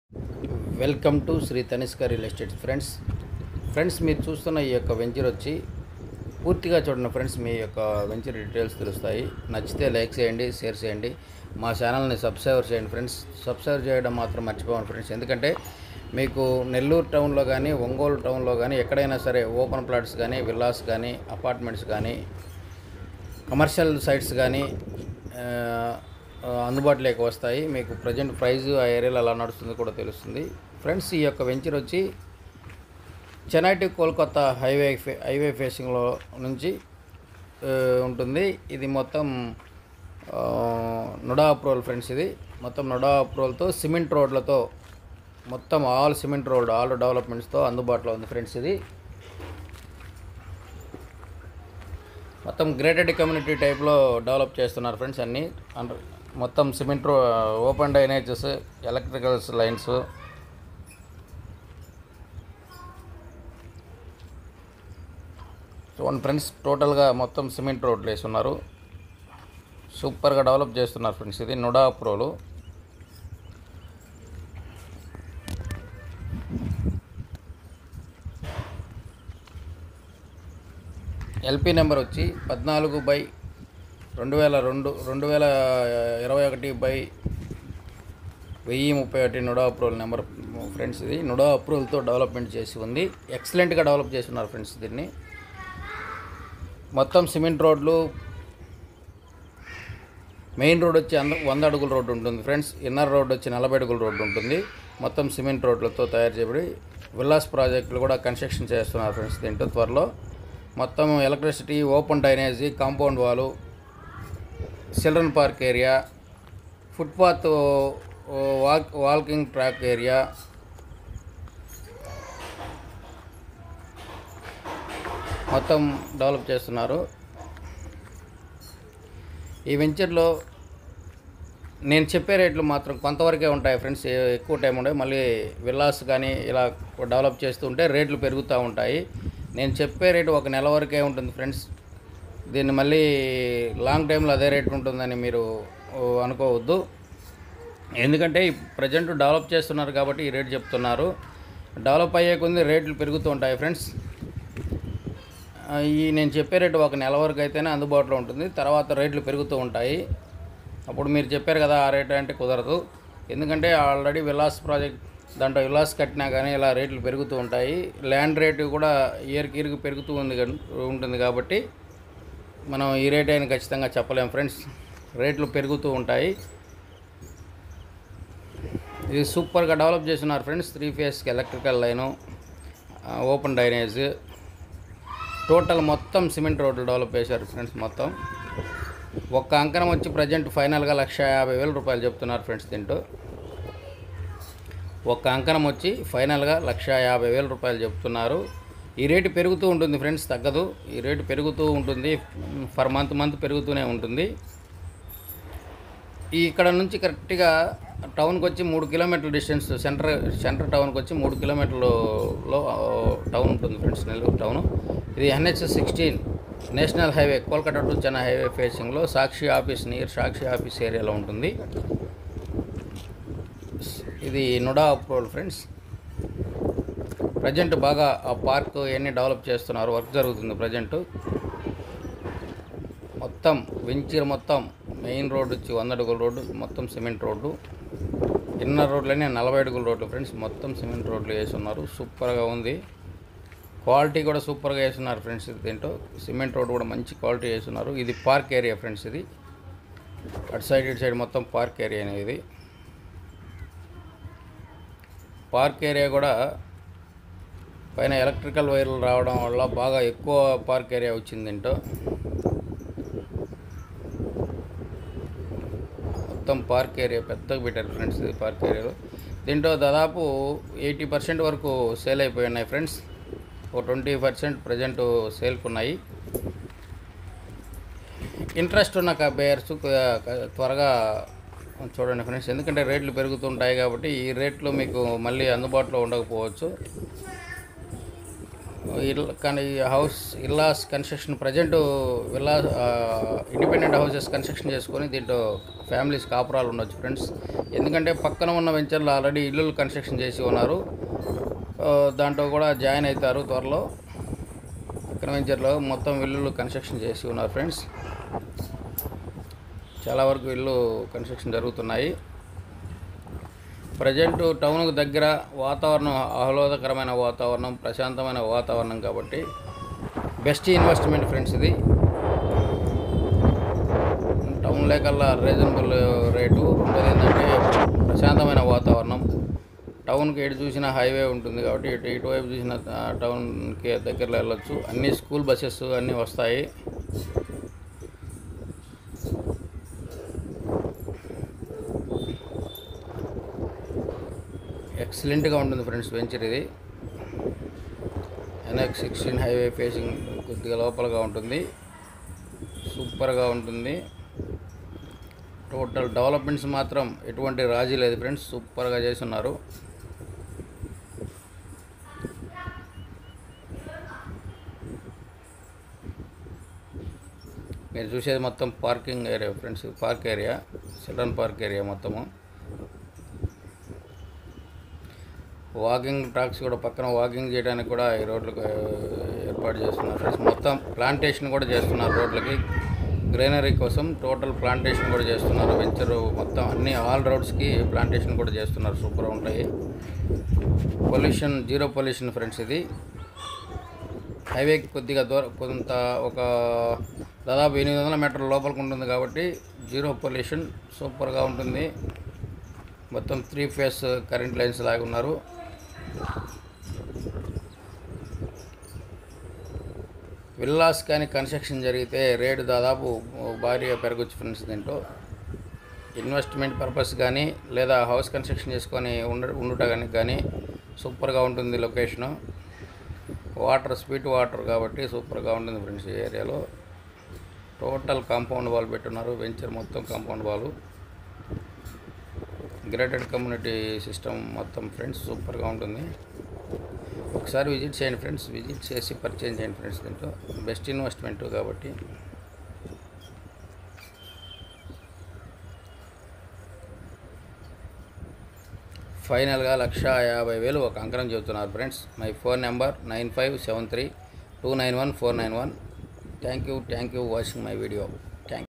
Nellore town Nh16 haiway facing nuda approval open plot for sale available full dowlpment area
Nellore Sakshi office back side nh16 haiway facing nuda approval layout lo open plot for sale available

main road 100 ft

iner road 40 ft  cement road s

Commercial plot& villa s plot s

electricity

Open drainage

compound wall around the layout

Avenue plantation

 foot path & walking track

children parking  playing area

3 km distance Nellore town  center

 the best investment open plot

what app no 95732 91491